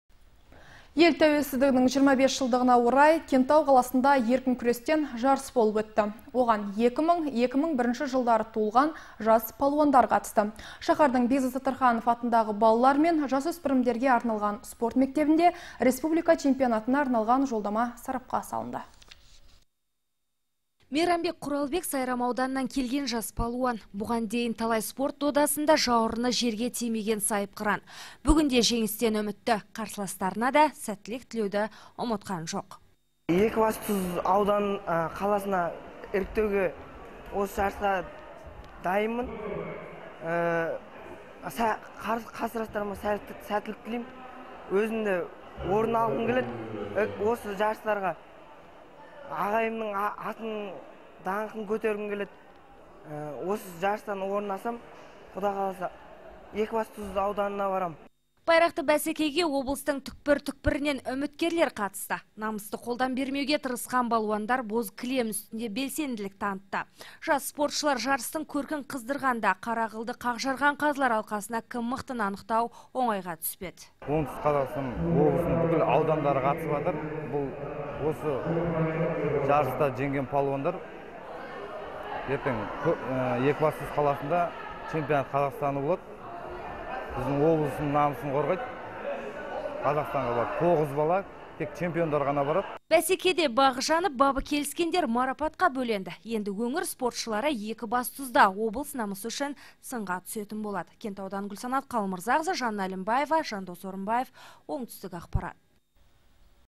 Еркін күрестен ел чемпионатына қатысушылар іріктелді
Ел тәуелсіздігінің  25 жылдығына орай Кентау қаласында еркін күрестен жарыс болып өтті.  Оған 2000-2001 жылдары туылған жас палуандар қатысты.